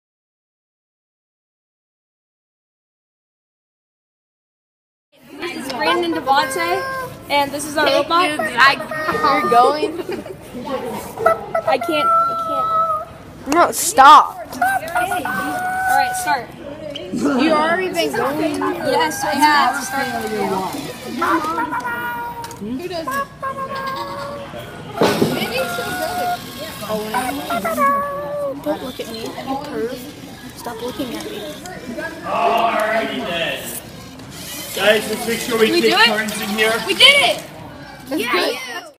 5DavonteBraden
There were 4 different search and rescue challenges. This robot had to clear debris out of the way so a rescue vehicle get through.